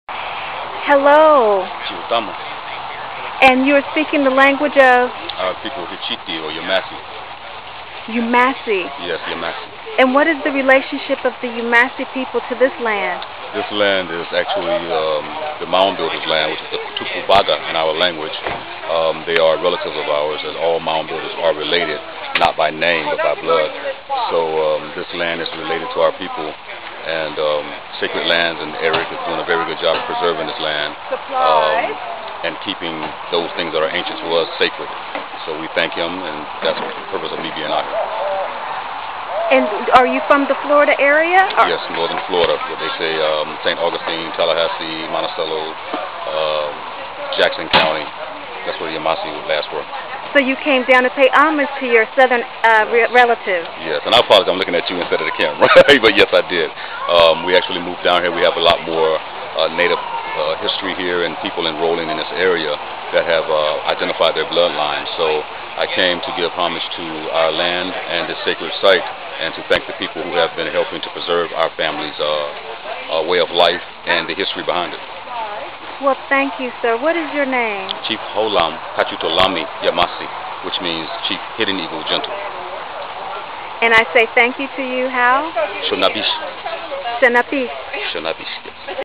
Chief Se'khu Holam Hatchitulamiy Gentle of YAMASSEE NATION
The Chief introduces himself during a recent visit to St. Petersburg, Florida down from Tallahassee to visit his Tocobagan cousin's mound and to pay homage to those who steward it today. — at Sacred Lands Education and Preservation .